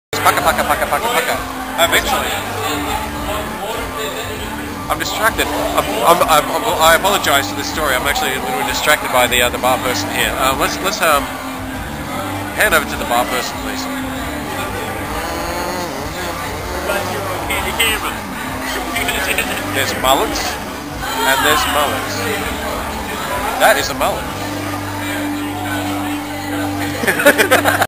... and then there's mullets
Tim getting distracted by a glorious mullet in Goulburn (13/11/09)